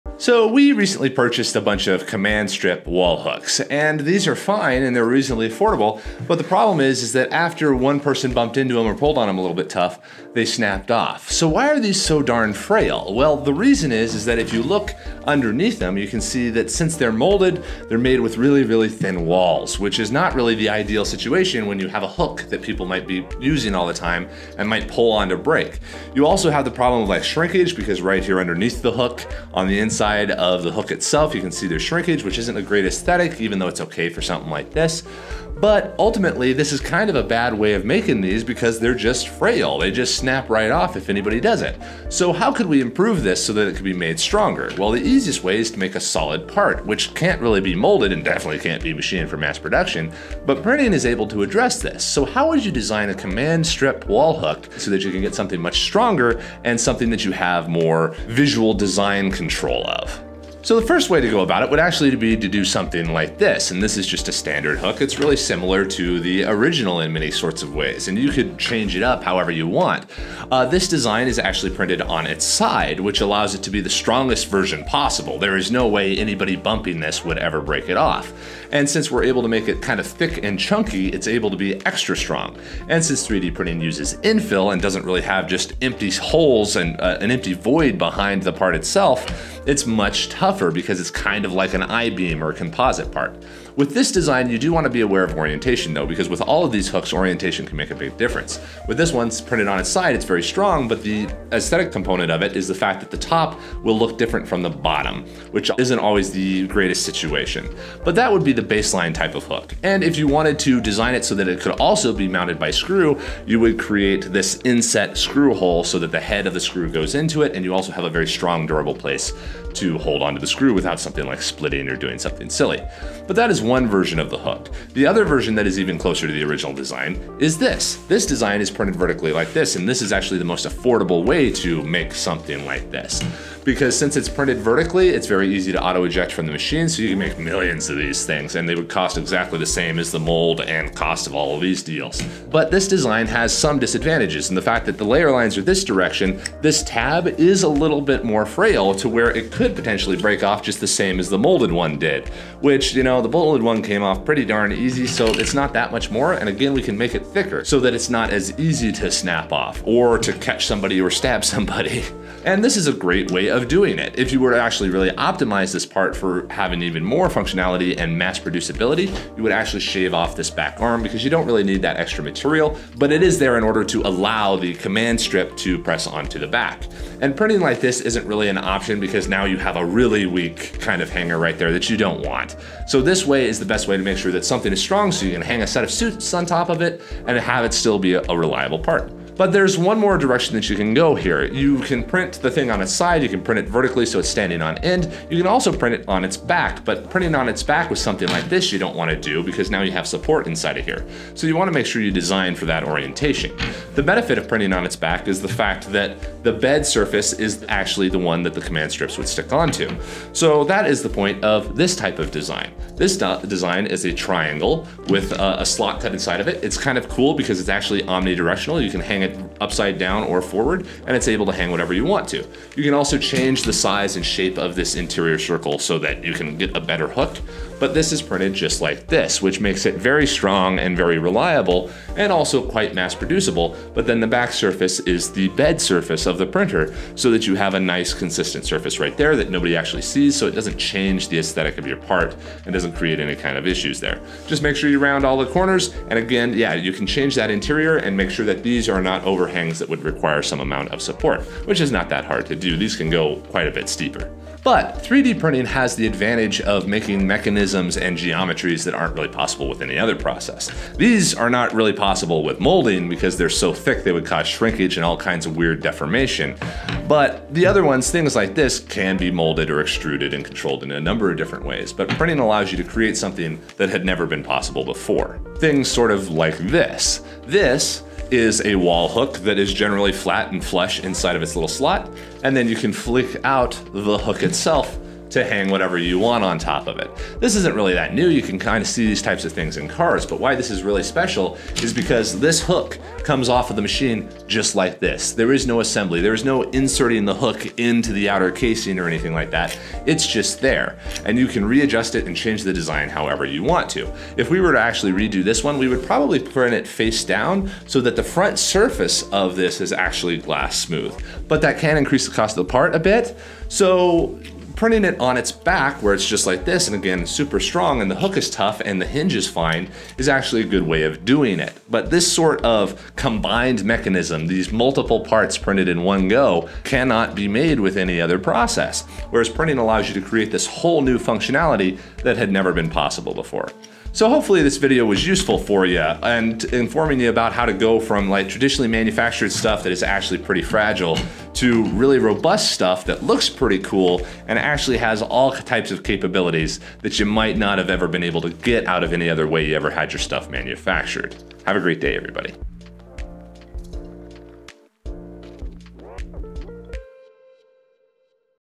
Command Strip Wall Hooks | Design for Mass Production 3D Printing
In this episode of Design for Mass Production 3D Printing, we explore 3D printed wall hooks!

Ever been frustrated by how fragile store-bought Command Strip wall hooks can be? We delve into the engineering behind their design, and more importantly, how we can create custom designs optimized for 3D printing. We discuss the benefits of Mass Production 3D Printing and how you can leverage these benefits to design hooks that are not just stronger but also aesthetically pleasing. Whether you're in need of a wall hook or just curious about design for 3d printing, this video promises insights and solutions. Say goodbye to frail hooks and hello to the future of home accessories!

00:00 Why Are Command Strip Wall Hooks So Frail?
01:00 Standard Wall Hook Design
02:04 Vertical Wall Hook Design
03:09 Triangular Wall Hook Design
04:14 Hinged Wall Hook Design